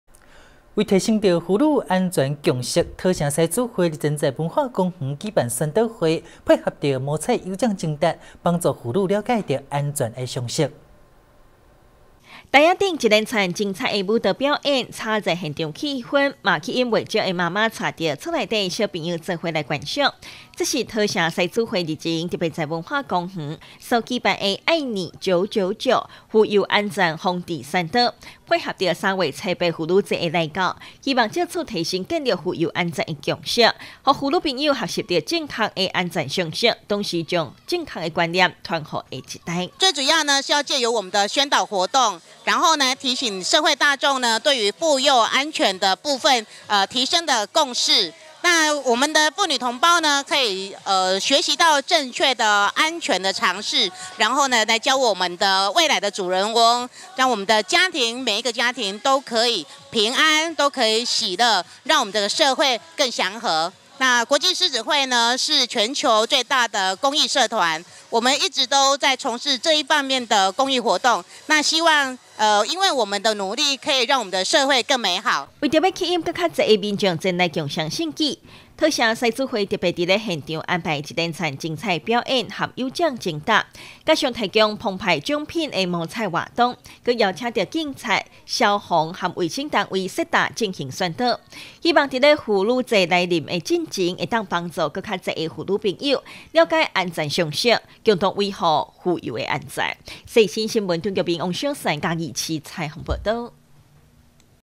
世新新聞 愛你久久久 桃城獅子會宣導婦幼安全
106年3月06日(一)世新新聞

愛你久久久 桃城獅子會宣導婦幼安全

為了增進婦幼安全常識，提昇婦幼安全之共識，桃城獅子會日前特別在文化公園，舉辦「愛你久久久 婦幼安全防治宣導」，現場安排一連串精彩的表演與有獎徵答，配合摸彩活動，更邀請到警察、消防及衛生單位設攤進行宣導，幫助婦女了解安全常識，共同維護婦幼的安全。